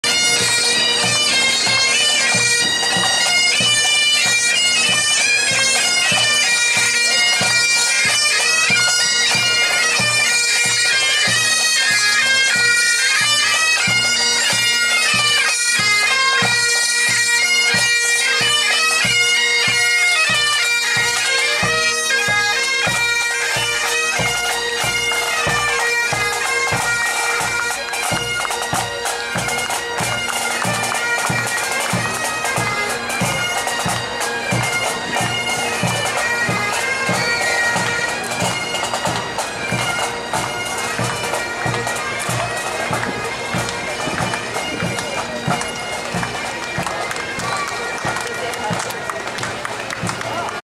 48th Highlanders of Canada Marching in the 2023 Warriors' Day Parade
The 48th Highlanders are seen in this video participating in the 2023 Warriors' Day Parade at the Canadian National Exhibition (CNE) on August 19th, 2023.